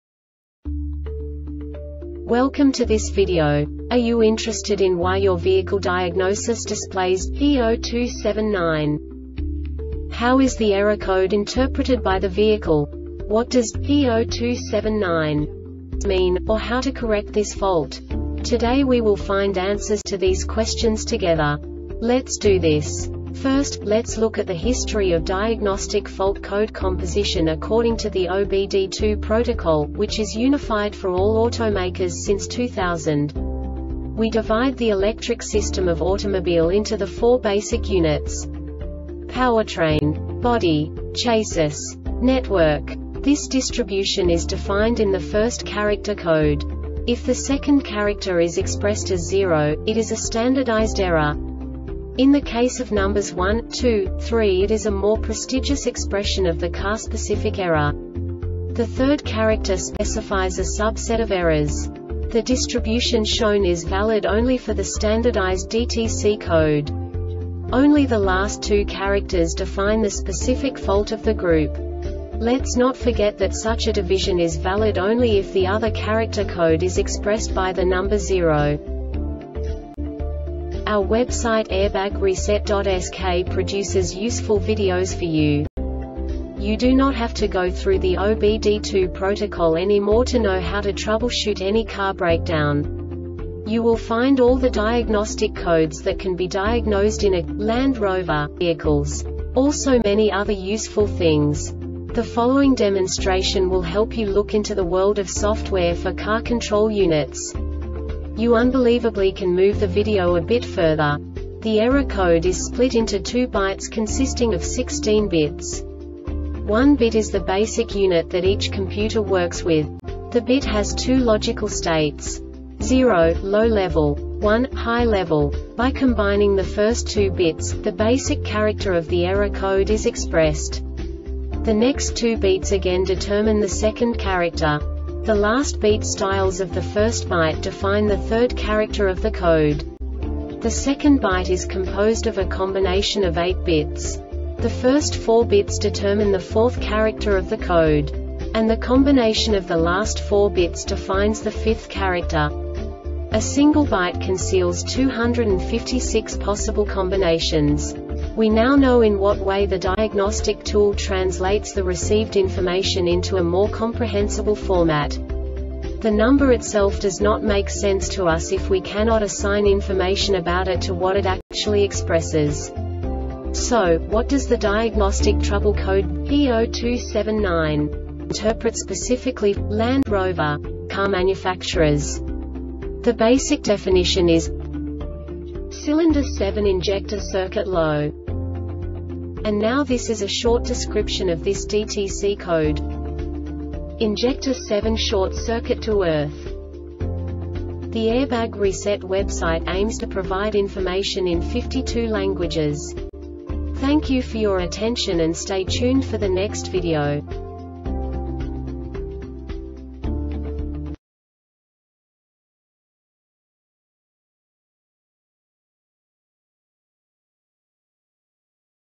DTC Landrover P0279 Short Explanation
Contents:
0:21 Basic DTC analysis according to OBD2 protocol standard.
1:48 Insight into programming
2:58 A diagnostically understandable description of the Diagnostic Trouble Code
3:05 Basic error definition

Make
Land Rover
Code
P0279
Definition
Cylinder 7 injector circuit low
Description
Injector 7 short circuit to earth